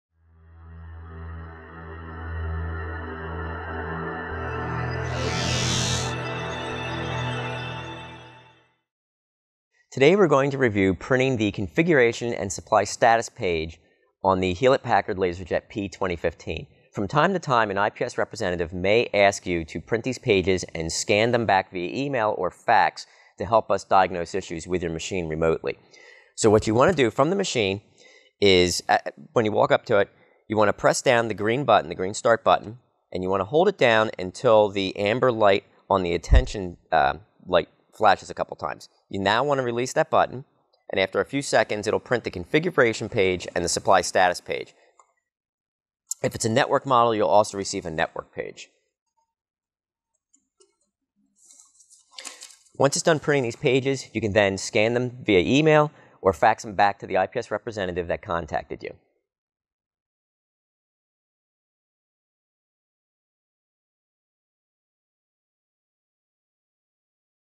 HP P2015 - Printing the Configuration & Supply Status Pages
This video from IPS global explains how to print the configuration page and the supply status page on the HP P2015 LaserJet Laser Printer.